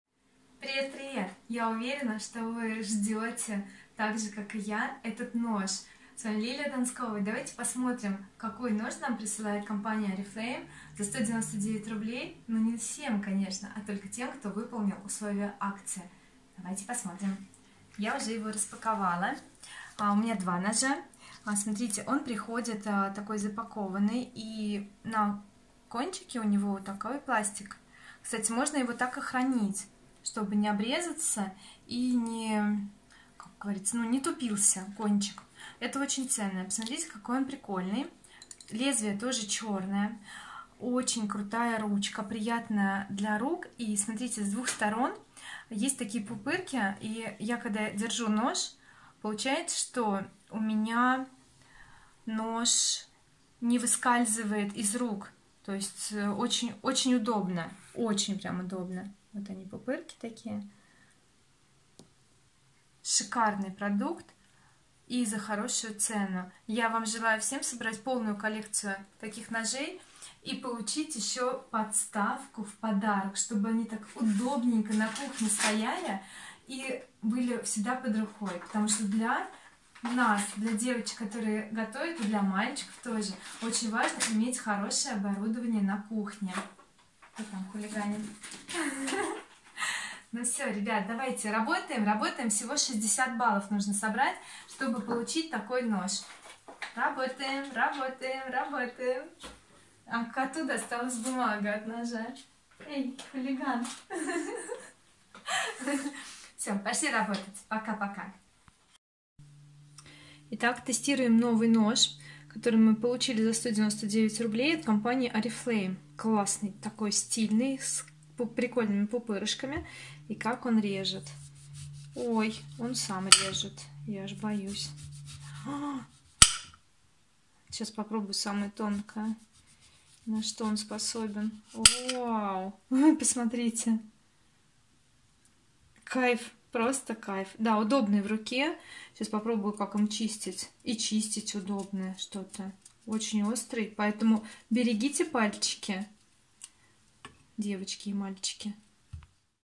ОБЗОР УНИВЕРСАЛЬНОГО КЕРАМИЧЕСКОГО НОЖА по акции Oriflame
Обзор универсального керамического ножа (код 526163) по акции Oriflame "Рецепт успешной вечеринки".